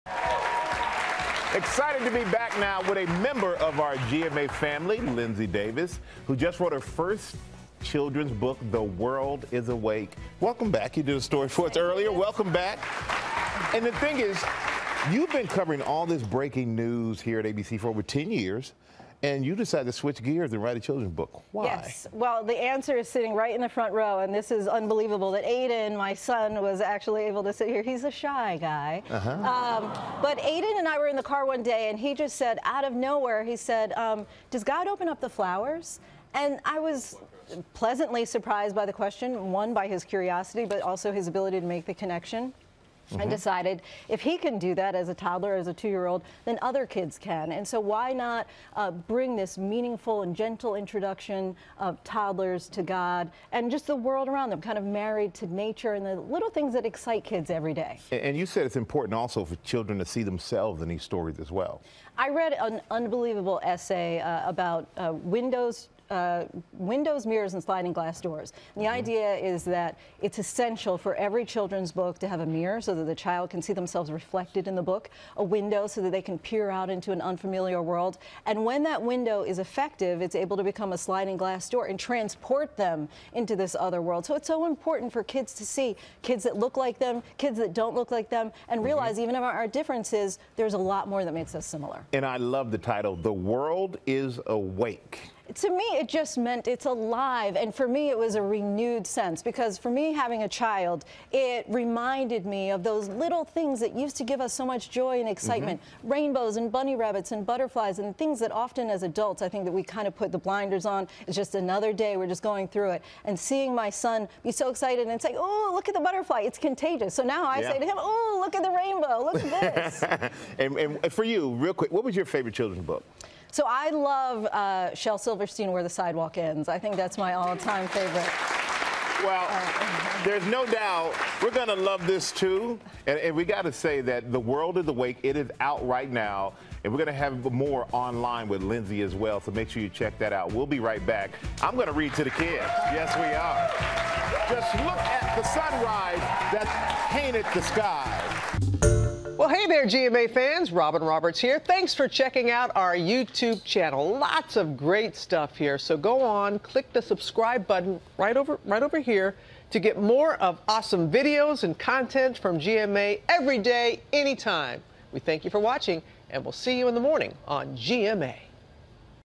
ABC News' Linsey Davis opens up about her inspiring new children's book
The ABC News correspondent's new book, "The World is Awake: A Celebration of Everyday Blessings," aims to inspire children to be thankful.